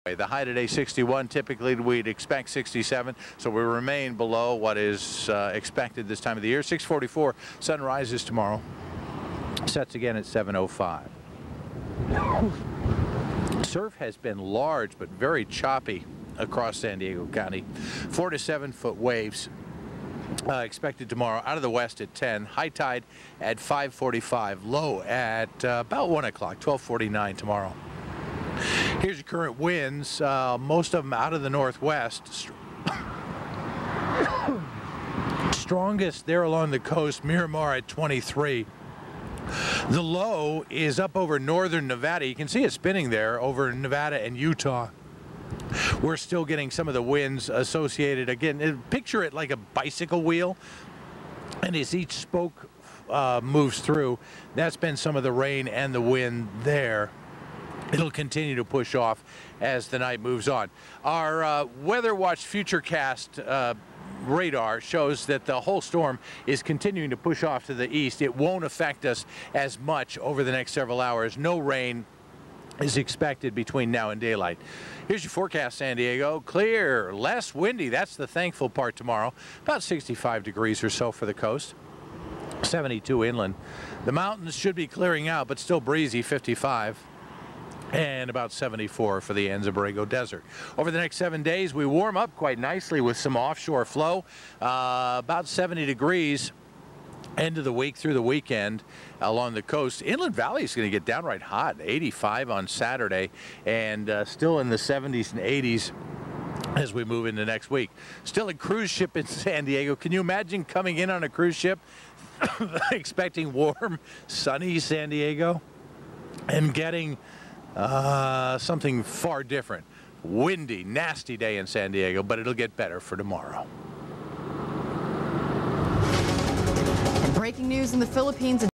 KGTV Weatherman Coughing Fit
Loren Nancarrow gets a terrible frog in his throat during a live weather broadcast.  Listen to how painful it sounds when he tries to talk while supressing the coughing.